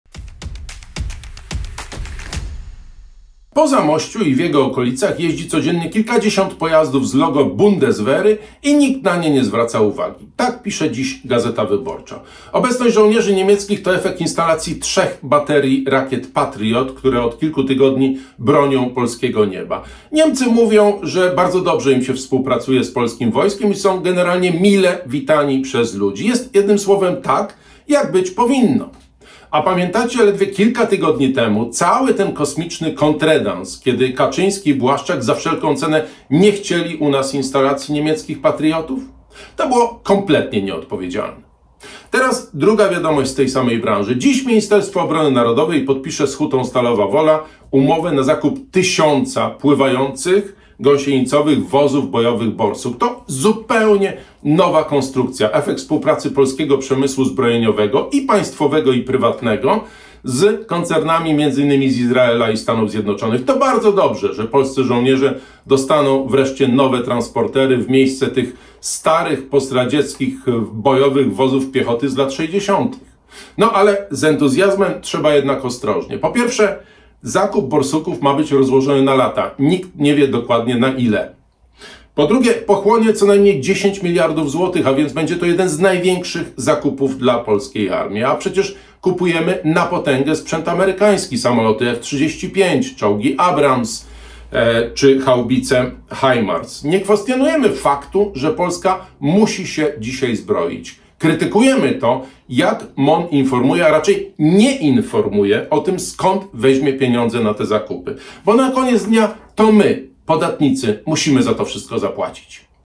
28.02.2023| DwM: Po Zamościu i w jego okolicach jeździ codziennie kilkadziesiąt pojazdów Bundeswehry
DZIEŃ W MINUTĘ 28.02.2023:
Po Zamościu i w jego okolicach jeździ codziennie kilkadziesiąt pojazdów
Bundeswehry i nikt na to nie zwraca uwagi, pisze dziś „GW”. Obecność żołnierzy niemieckich wiąże się z instalacją trzech baterii rakiet Patriot, które od kilku tygodni bronią polskiego nieba.
Komentarz dnia publikowany przez przedstawicieli Ruchu Polska 2050.